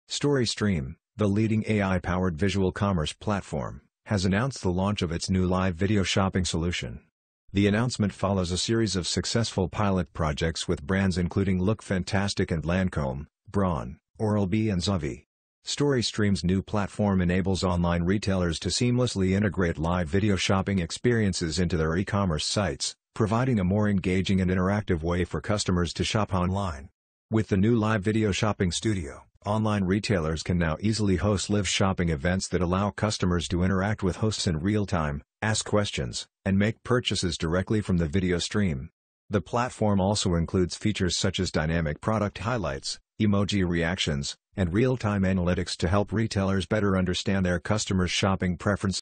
StoryStream Launches New Live Video Shopping Platform for Online Retailers
StoryStream, the leading AI-powered visual commerce platform, has announced the launch of its new Live Video Shopping solution. The announcement follows a series of successful pilot projects with brands including LookFantastic & Lancome, Braun, Oral-B and Zavvi.

StoryStream’s new platform enables online retailers to seamlessly integrate Live Video Shopping experiences into their eCommerce sites, providing a more engaging and interactive way for customers to shop online.

With the new Live Video Shopping Studio, online retailers can now easily host live shopping events that allow customers to interact with hosts in real-time, ask questions, and make purchases directly from the video stream. The platform also includes features such as dynamic product highlights, emoji reactions, and real-time analytics to help retailers better understand their customer's shopping preferences and behaviours.

GangnamToday News